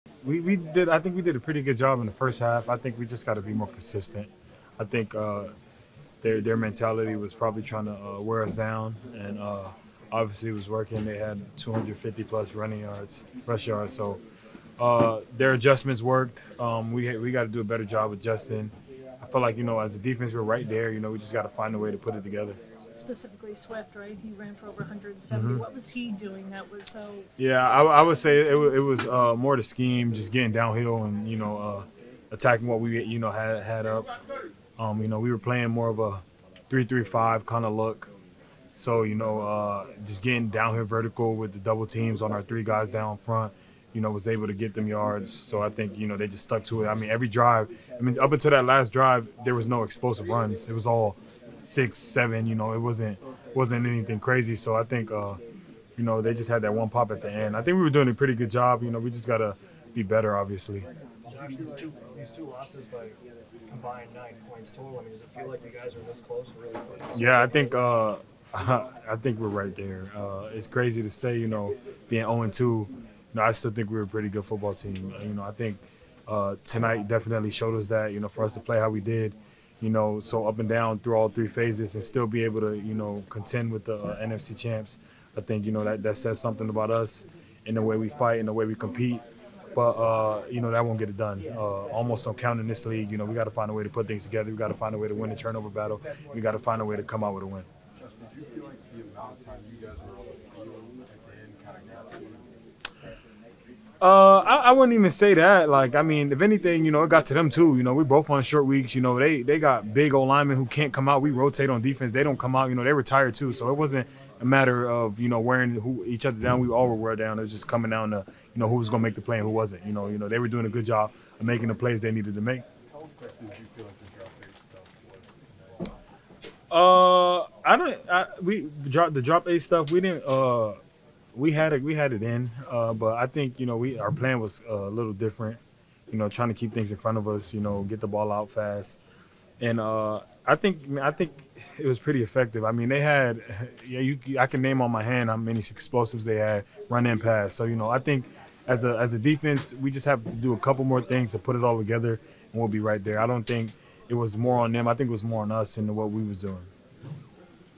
Josh Metellus: I Feel Like As A Defense We're Right There
Minnesota Vikings safety Josh Metellus addressed the media after the Week 2 game against the Philadelphia Eagles.